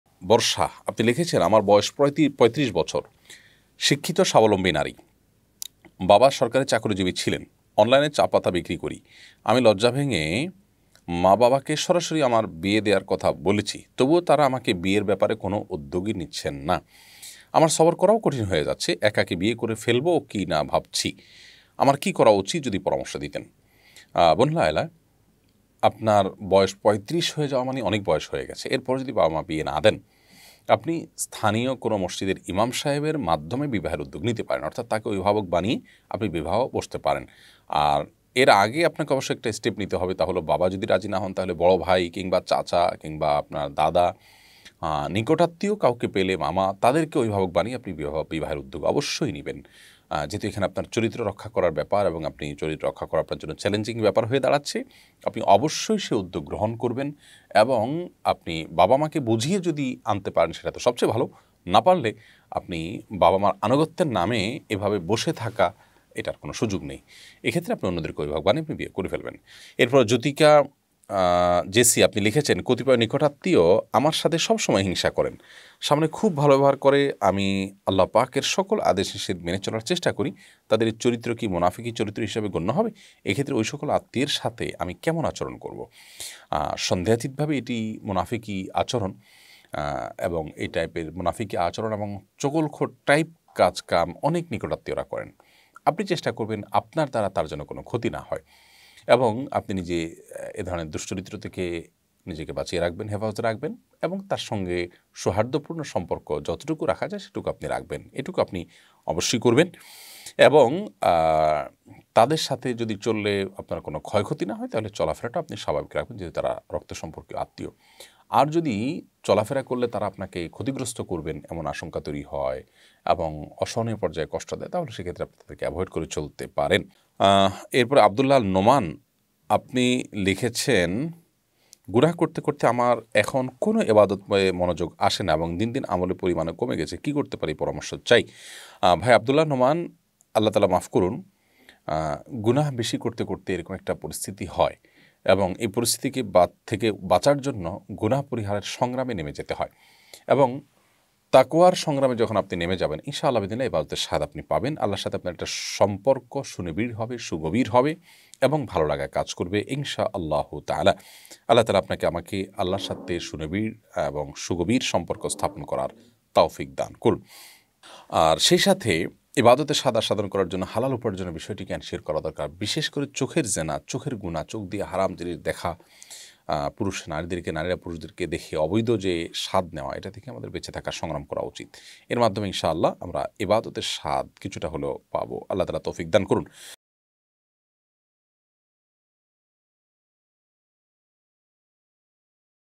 ৩৫ বছরের অবিবাহিত মেয়েকে যা করতে বললেন শায়খ আহমাদুল্লাহ! ইসলামী প্রশ্নোত্তর II Sheikh ahmadullah
৩৫ বছরের অবিবাহিত মেয়েকে যা করতে বললেন শায়খ আহমাদুল্লাহ! ইসলামী প্রশ্নোত্তর II Sheikh ahmadullah live